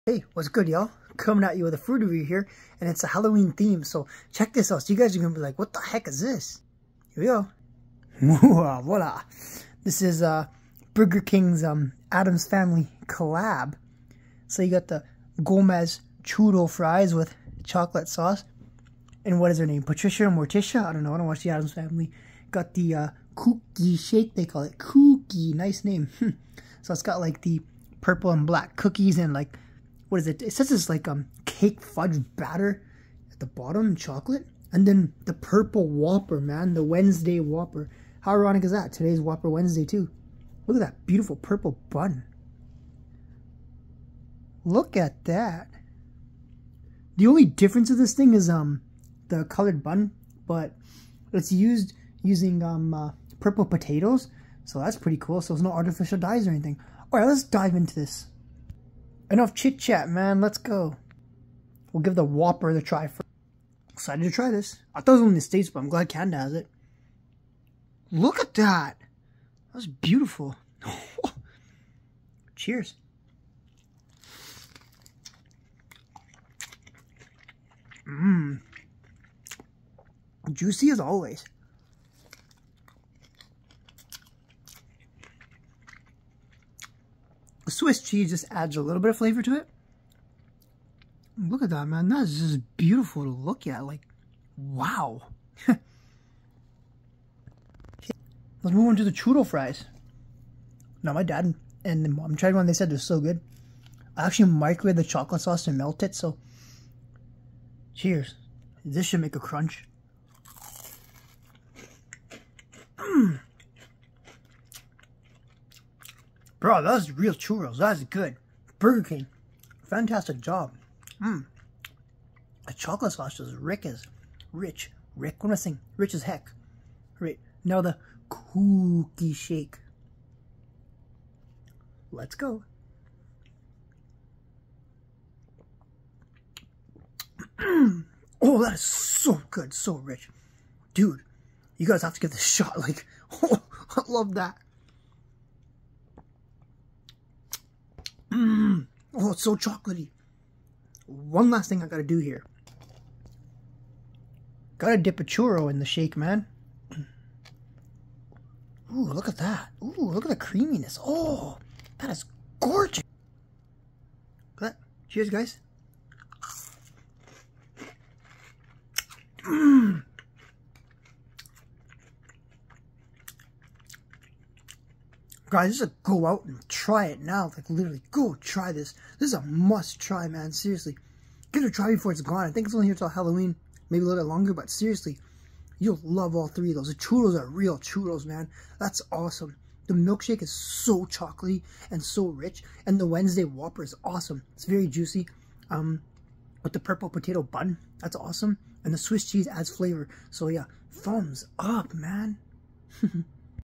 Burger Kings Addams Family Collab
Behold, Burger King X Addams Family collab. I've got the Wednesday's whopper, Gomez's churo fries, and Morticias Kooky Chocolate Shake. I kid you not, the desserts are so chocolaty, and they are a chocolate lovers dream. These are a must try, I promise you, you will be very happy. The purple bun Is so beautiful to look at.